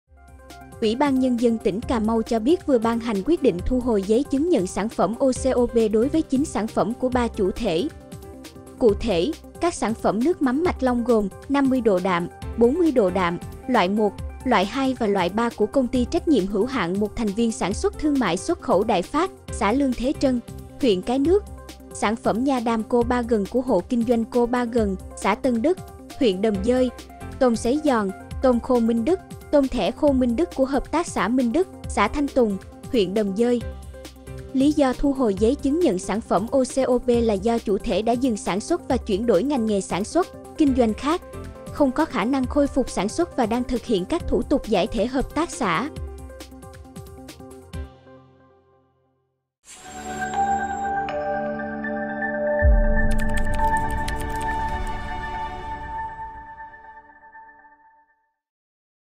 Cà Mau: Thu hồi giấy chứng nhận 9 sản phẩm OCOP
UBND tỉnh Cà Mau cho biết vừa ban hành quyết định thu hồi giấy chứng nhận sản phẩm OCOP đối với 9 sản phẩm của 3 chủ thể.